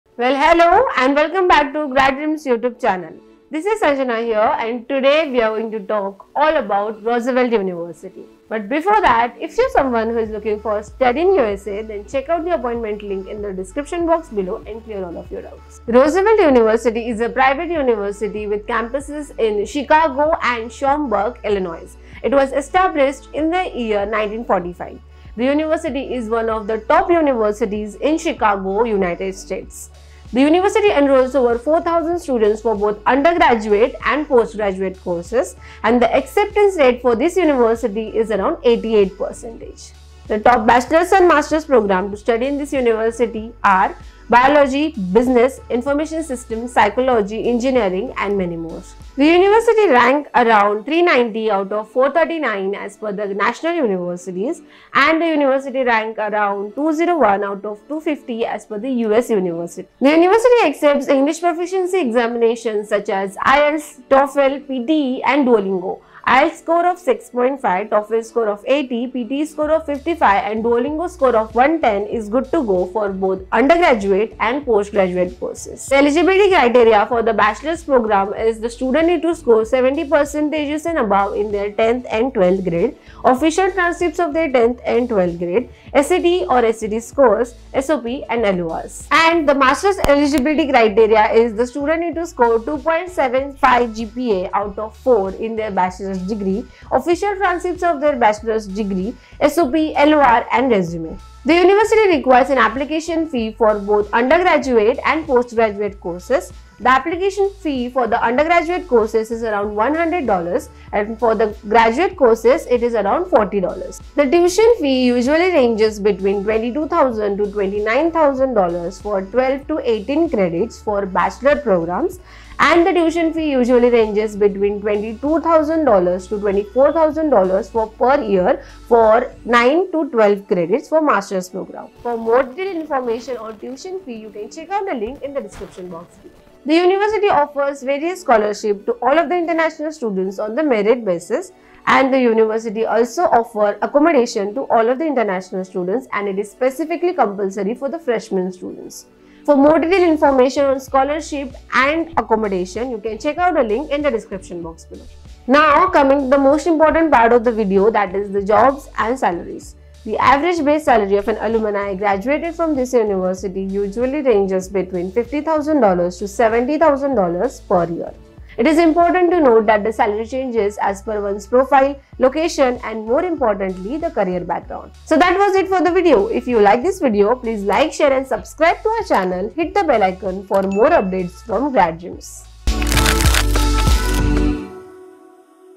Study in USA at Roosevelt University for FALL 2024
✅Unlock your academic dreams in the heart of the USA! Discover the opportunities awaiting you at Roosevelt University. Experience the American Dream at Roosevelt University – where education meets excellence. Your journey to success begins here! Join us at Roosevelt University and study in the USA. Grad-Dreams help you choose the right career, course, and country for your education abroad.

00:00 Welcome Back to Grads YouTube Channel 🎥
00:02 All About Roosevelt University 🏫
00:22 Top Universities in Chicago 🌆📚
00:30 Over 4,000 Students Enrolled 🎓
00:36 High Acceptance Rate (88%) 👍
00:55 University Ranking: 2011 out of 250 in US 🌍
01:07 English Proficiency Exams Accepted 📝🌐
01:13 Good Scores for Undergrad and Postgrad 🎓
01:31 GPA of 2.75 Needed for Master's ✔️
01:38 Official Transcripts, Sop, LOR, Resume Needed 👨‍🎓
01:46 Application Fee: $100 for Undergrad, $40 for Grad 💰
02:18 Scholarships for International Students 💼💰
02:26 Accommodation Compulsory for Freshman Students 🏢
02:38 Average Salary Range: $50,000 to $70,000 per year 💵
02:56 Like, Share, and Subscribe for More Updates 🎉🔔